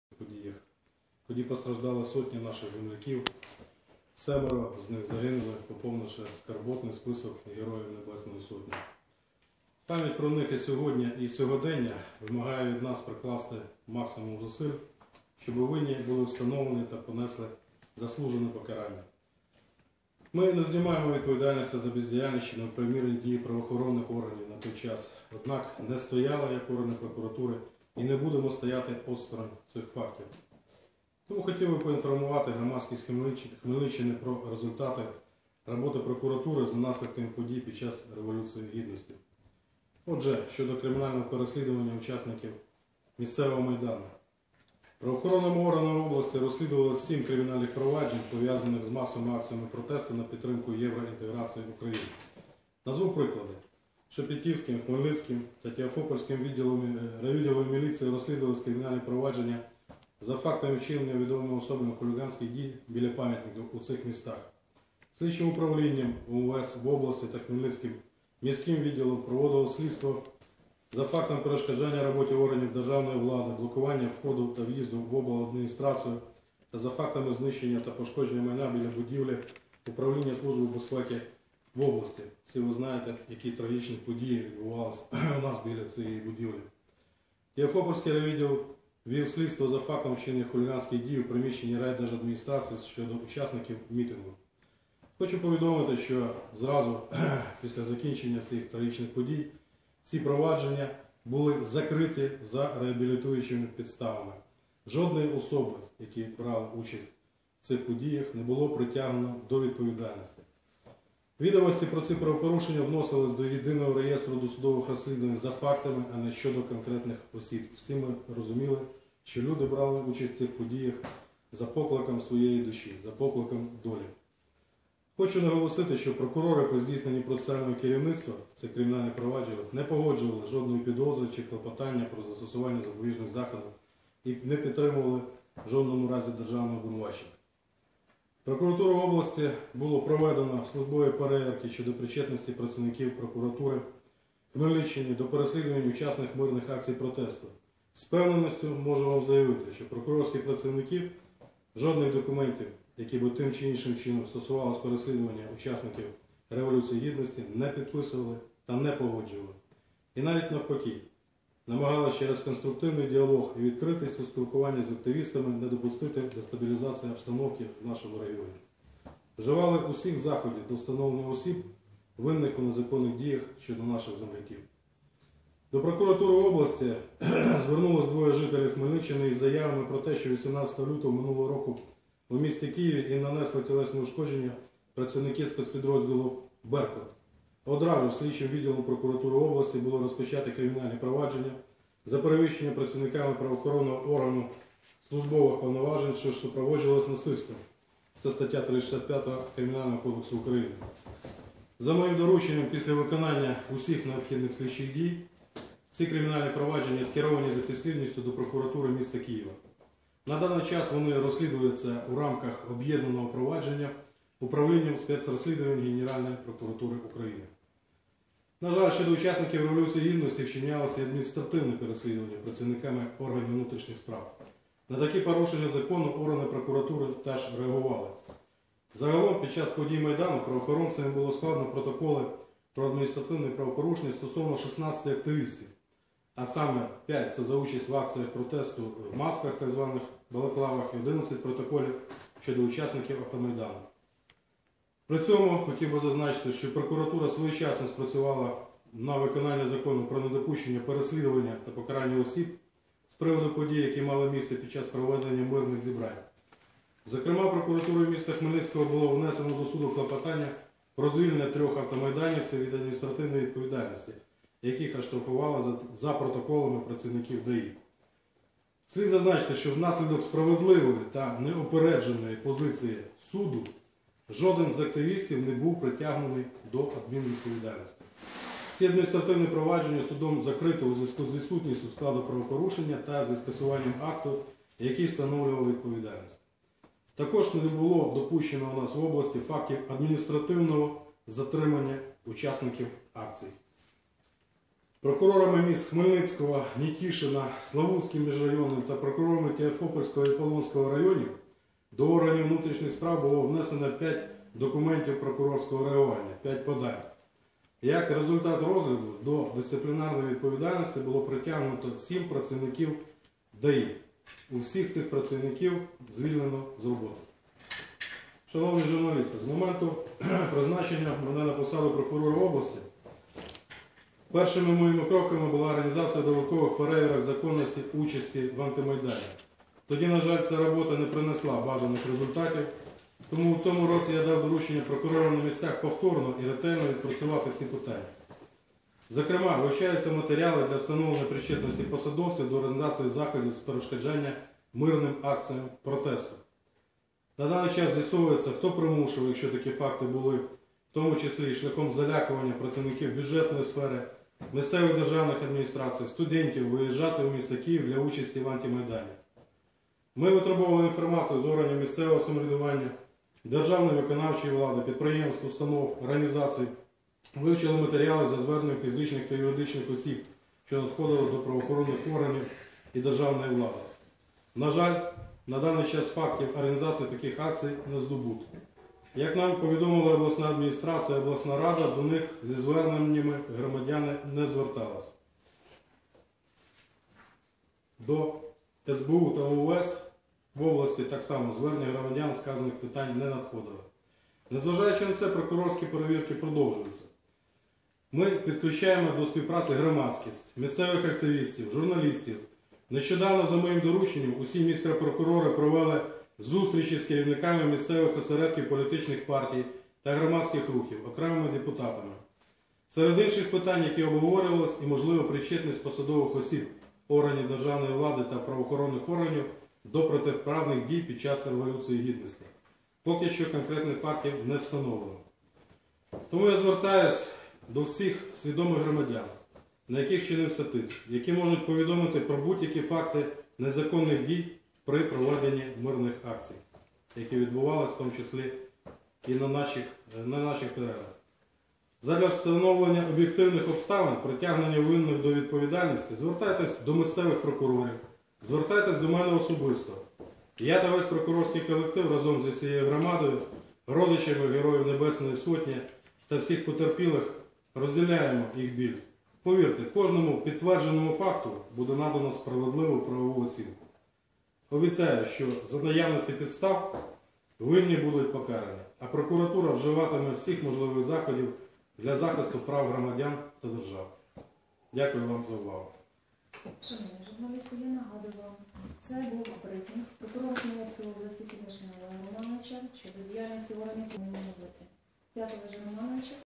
Прокурор Хмельницької області про розслідування злочинів під час Революції Гідності
Прокурор Хмельницької області Олег Синишин розповів про зловживання влади та правоохоронних органів під час Революції Гідності.